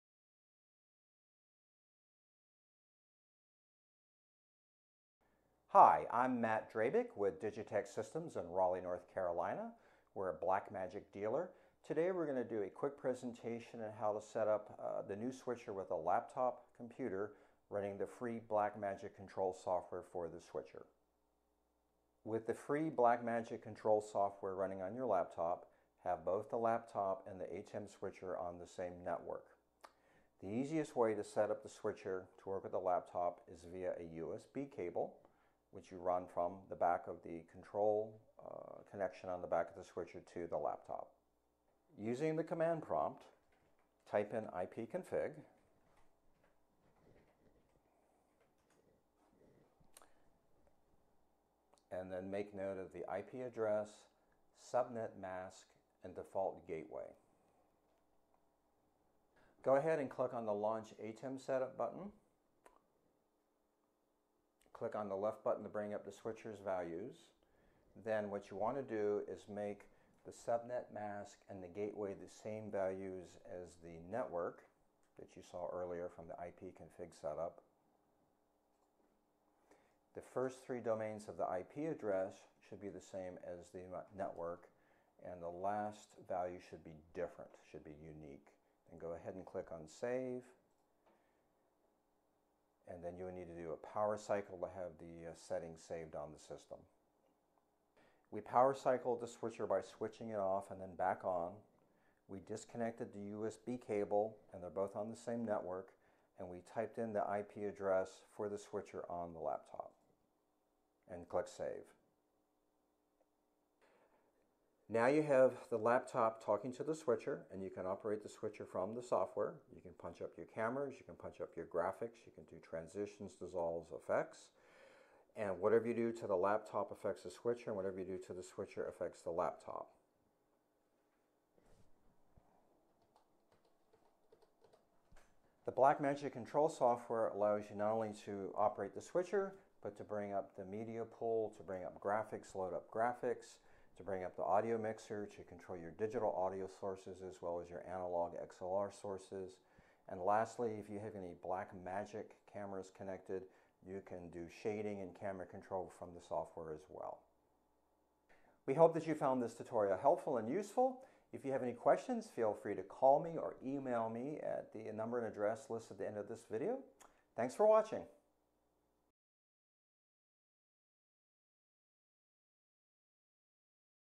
How to connect ATEM Control Software to the Blackmagic ATEM Television Studio Pro HD
DigiTekSys.com